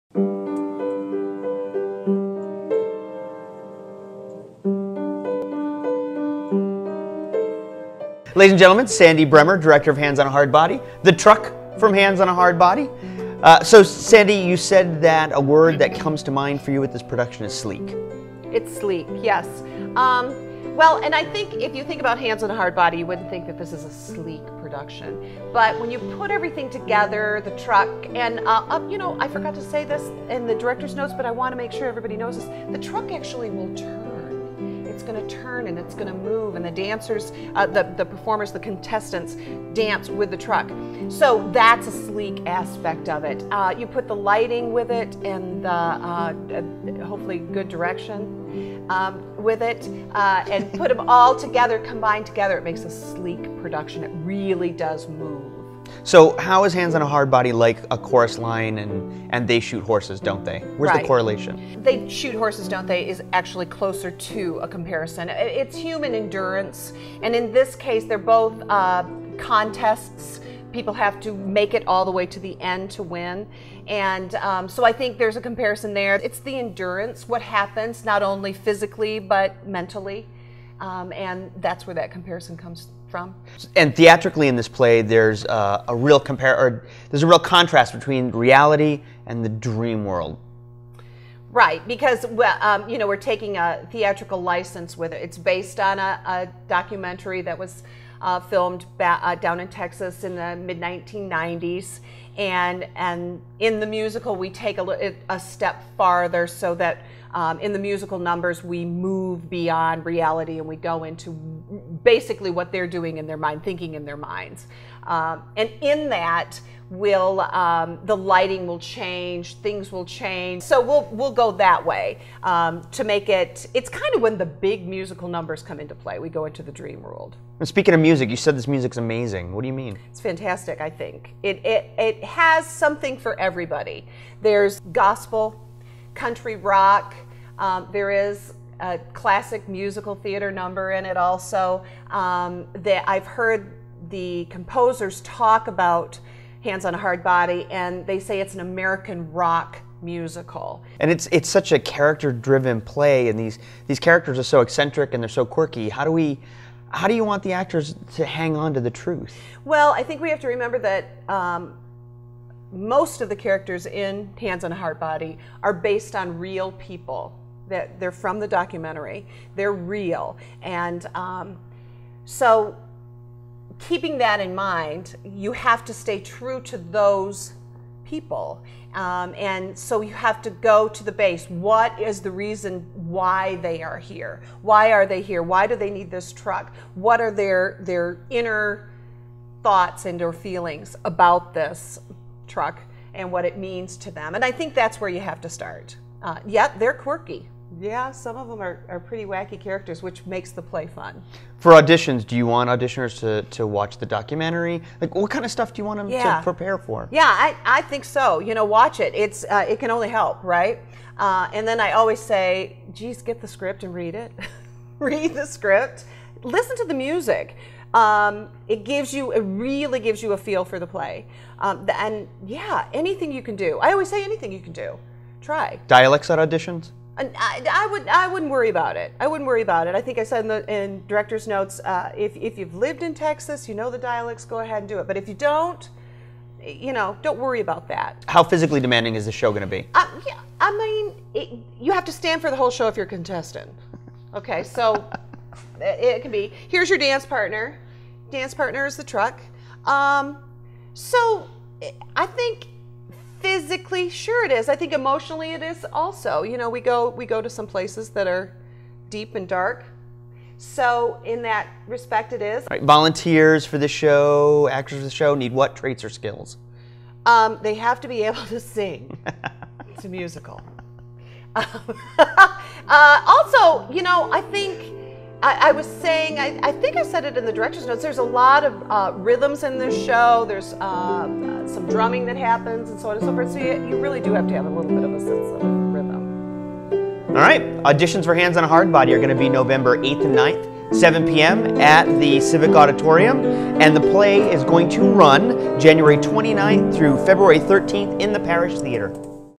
Hands On A Hardbody - We Asked The Director
Sandra Bremer, Director of The Kalamazoo Civic Theatre's upcoming production of Hands On A Hardbody, discusses the production including what volunteers can expect at auditions and the rehearsal process.   Hands On A Hardbody auditions Sunday, November 8, 2015 and Monday, November 9, 2015 at 7:00 PM in The Civic Auditorium - Rehearsal Room (329 S Park St, Kalamazoo, MI 49007).   For more audition information or to register in advance email or call . The musical plays in the Parish Theatre January 29, 2016 through February 13, 2016. or call .